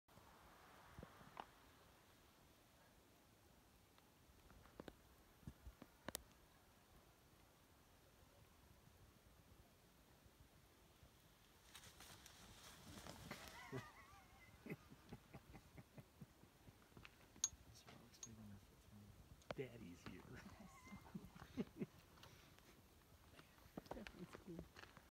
Condor Fly-in
A Condor flies in close at the top of Pinnacles National Park.